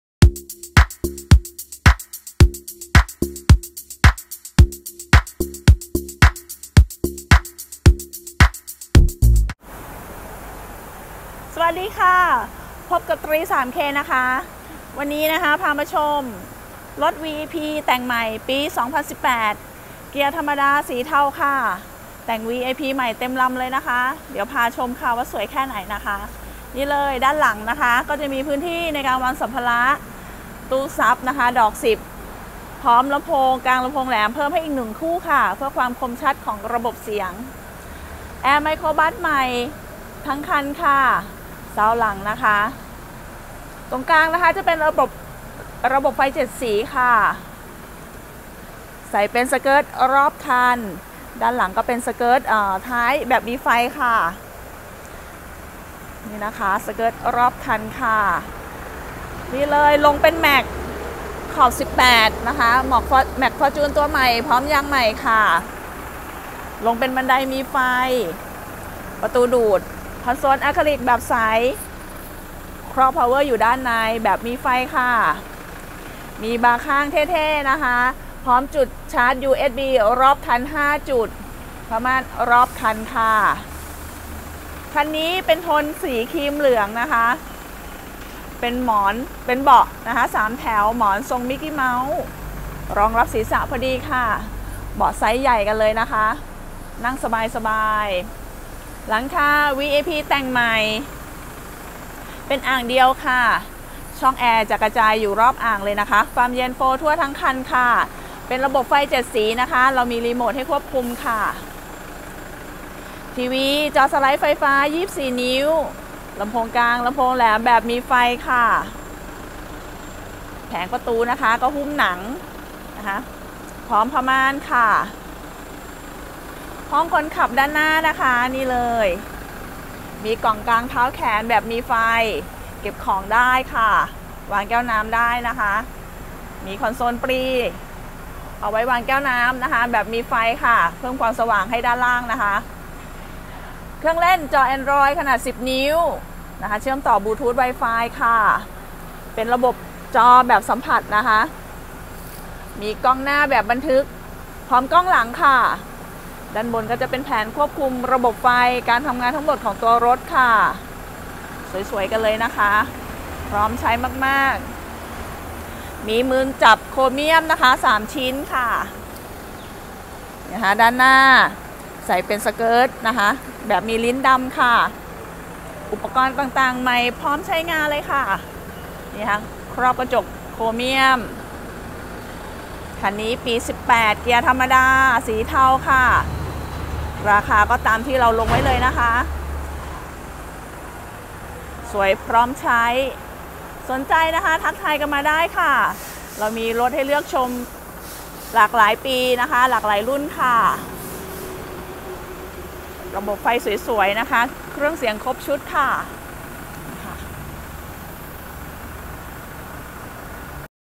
🚌💨รถเด่น งานดี VIP เต็มระบบ Toyota commuter 3.0ดีเซล ปี18 สีเทา เกียร์ธรรมดา
Toyota commuter 3.0ดีเซล ปี18 สีเทา เกียร์ธรรมดา
แต่งหลังคาใหม่วีไอพี
แอร์ใหม่ไมโครบัสหลัง
หุ้มข้างใหม่รอบคัน
หุ้มแผงประตูหน้า
คอลโซลเตี้ยใหม่
กล่องเก็บของ
ผ้าม่านทูโทน
เบาะใหม่วีไอพี9ที่นั่ง
ทีวีจอพับไฟฟ้า24นิ้ว
เครื่องเล่นแอนดรอย ชุดเครื่องเสียง ตู้ซับ
ล้อแม็กซ์ขอบ18 ยางใหม่
ประตูดูด
บันไดมีไฟ
กล้องบันทึกหน้า กล้องมองหลัง
เสกิร์ตรอบคัน

ดาวน์เริ่มต้น 249,000 บาท
ค่าจัดโอน 10,000 บาท
ผ่อนเดือนละ 17,xxx/72 เดือน

✍️นัดดูรถจริง ไม่ผ่านนายหน้า ตัวแทน ติดต่อ👇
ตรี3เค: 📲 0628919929

‼️ออกรถวันนี้  ขับฟรี 90 วัน
♦️ยอดจัดอาจเปลี่ยนแปลงไปตามเงื่อนไขของธนาคารและเครดิตของลูกค้า♦️